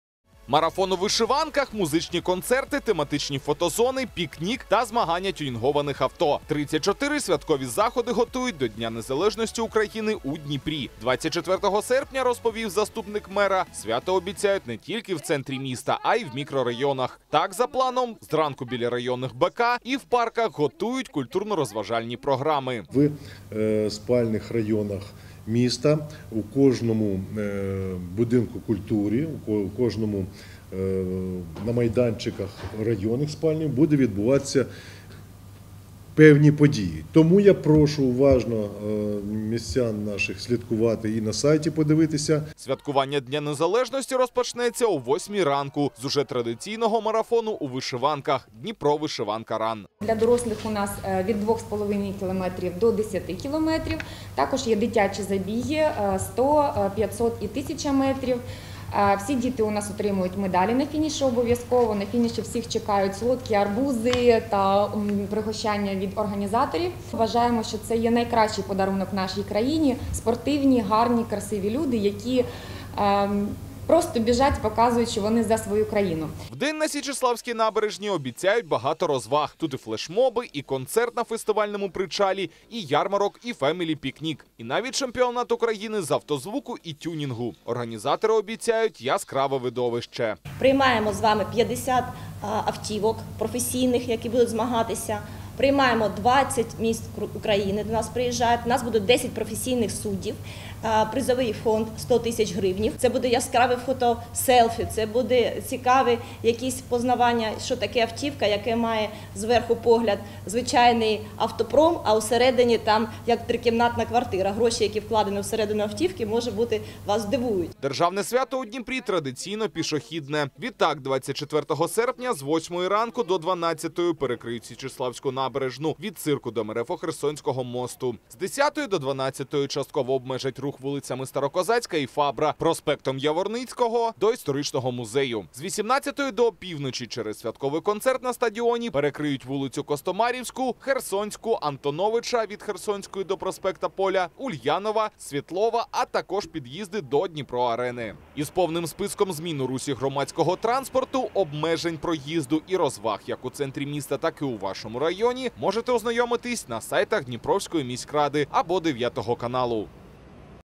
"Dnipro vyshyvanka run" розпочне День Незалежності в Дніпрі
Марафон у вишиванках, музичні концерти, тематичні фотозони, пікнік та змагання тюнінгованих авто - 34 святкові заходи готують до Дня Незалежності України у Дніпрі. 24 серпня, розповів заступник мера, свято обіцяють не тільки в центрі міста, а і в мікрорайонах. Так, за планом, зранку біля районних БК і в парках готують культурно-розважальні програми.